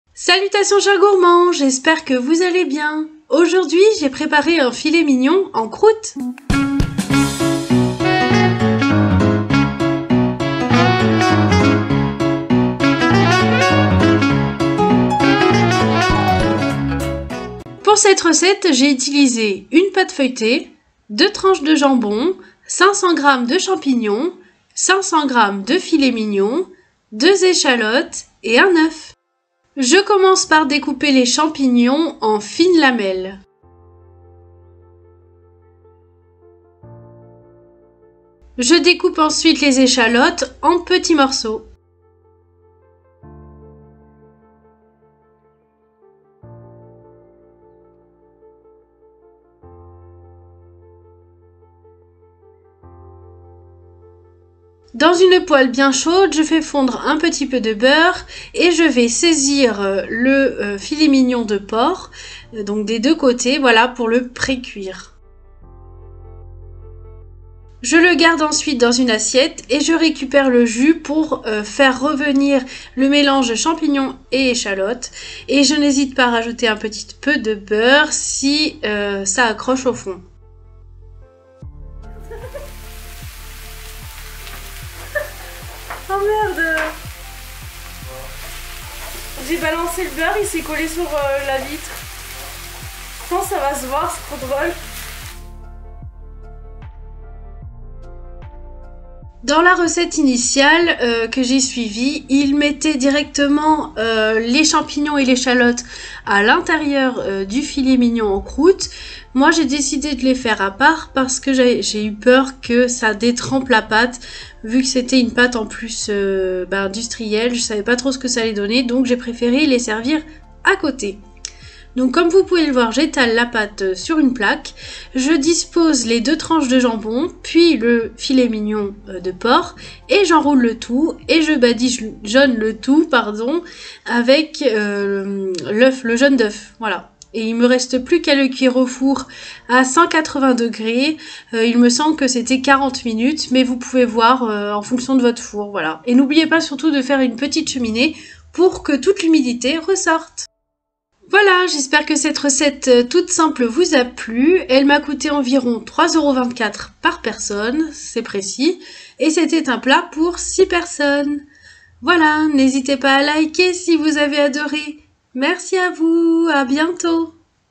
Filet mignon de porc en croute
Salutations chers gourmands !
Aujourd’hui, découvrez la recette d'un filet mignon de porc en croûte. J’espère qu’elle vous plaira.
Bonne dégustation ! A bientôt ! 😊

Je m’inspire des recettes proposées dans les magazines de cuisine tels que Gourmands, Saveurs, Sans gras ; et dans les blogs et sites internet comme 750g, Odelices, Cuisine AZ, Jow, MangerBouger.